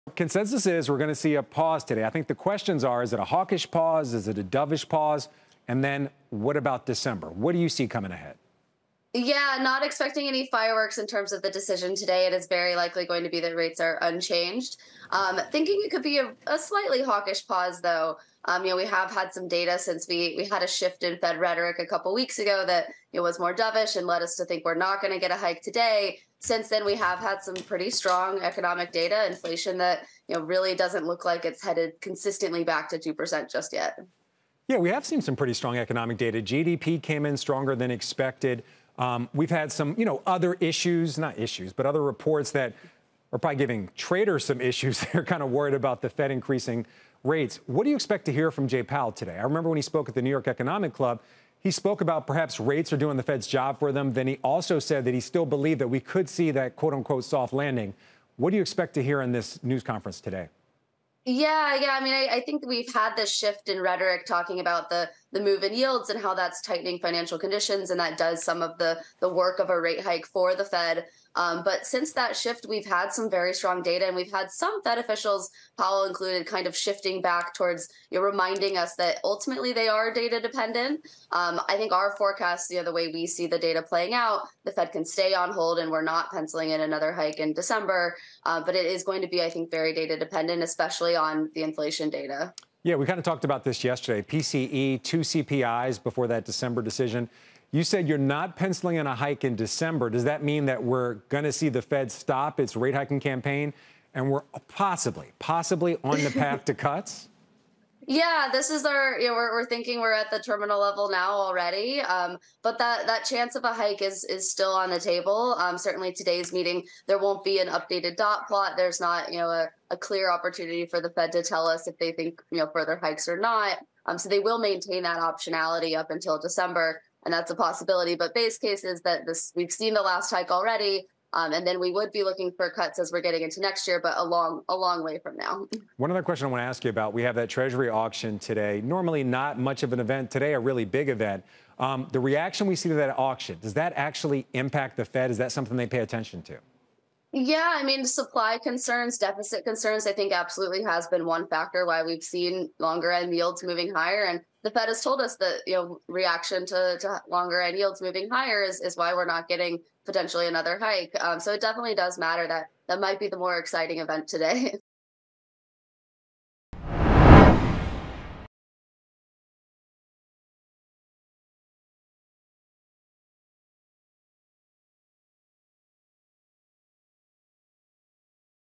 The chance of another rate hike is unlikely but still on the table, says Citi's Veronica Clark
Veronica Clark, Citi Economist, discusses the upcoming Federal Reserve monetary policy decision.